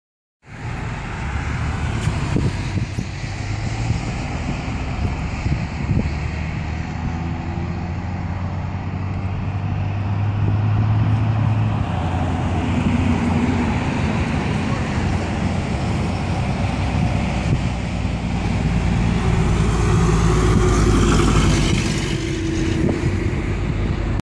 SUV Flipped In Car Crash
Vehicle flipped in accident . Interstate 82 and Elm Road (Walmart Exit)